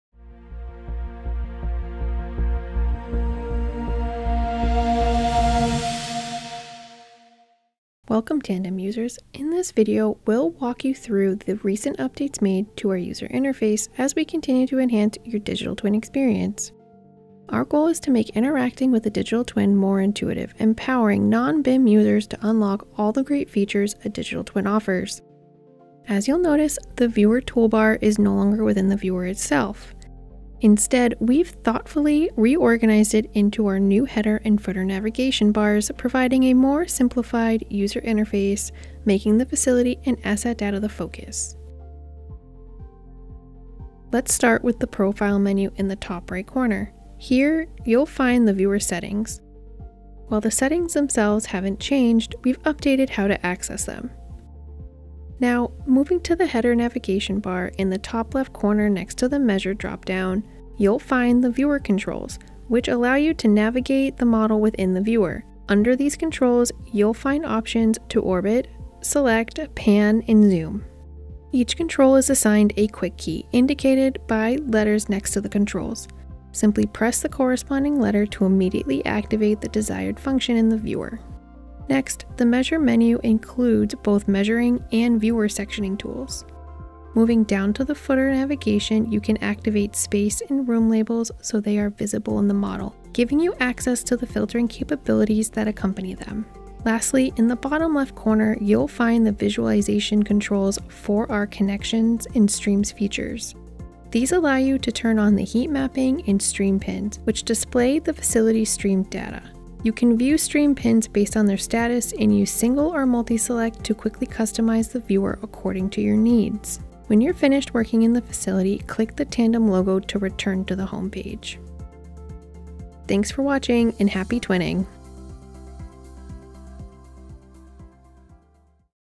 Simplified User Interface (UI)
Release Notes – Simplified UI

The Autodesk Tandem team is excited to announce a fresh UI for the Tandem facility experience as one of the first steps towards an easier interaction. Upon entering a facility, you will see a new header and footer and a viewer free from extra tools.

- New header and footer navigation bars when within a facility
- Relocated viewing, section, and measure tools from in-viewer toolbar to dedicated menus in header, at top right
- New navigation shortcuts to interact with the viewer, easily visible in the menus
- Relocated data visualization tools (labels, heatmap, stream pins controls) from in-viewer toolbar to new footer bar, left
- Relocated Properties panel toggle to footer bar, right
- Viewer Settings relocated to User Profile modal
- Clicking on Tandem logo takes you back to Homepage

Why does this matter? These updates are part of an initiative  aiming to make interacting with twin data simple and more intuitive. Everyone, not just BIM experts, should be empowered with digital twins and the rich contextual data they provide.